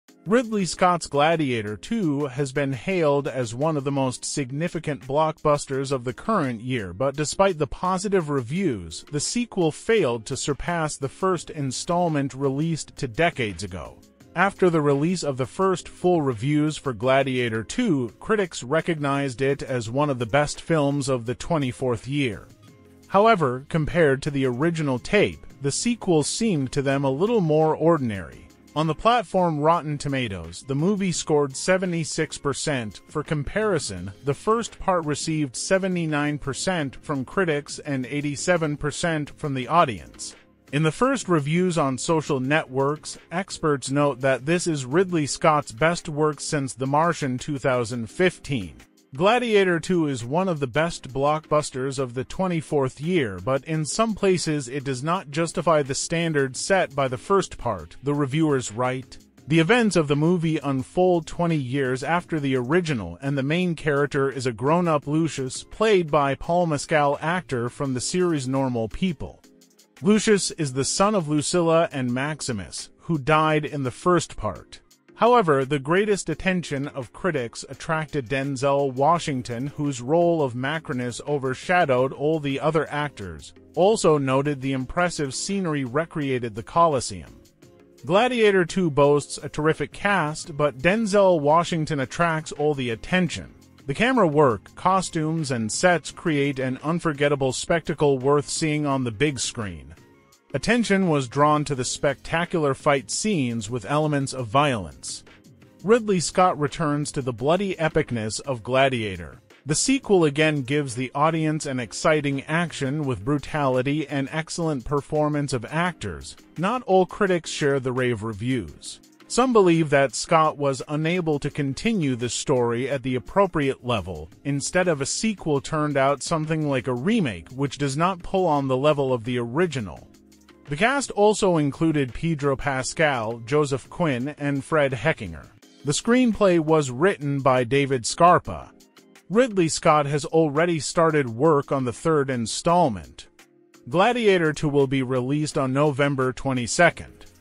Ridley Scott's “Gladiator 2” movie has been hailed in reviews as the best blockbuster of 2024
*Gladiator 2* is back on screens and all eyes are on this epic sequel. In this video, we take an in-depth look at what to expect from Ridley Scott's new movie. Despite the colorful scenes and amazing acting, many critics feel that the sequel can't outshine the original. Find out how spectacular fights, beautiful sets, and performances by Denzel Washington and Paul Mescal put the movie at the center of the debate. What did “Gladiator 2” surprised the audience and what flaws did it leave? Review on new events, characters and critics' opinion!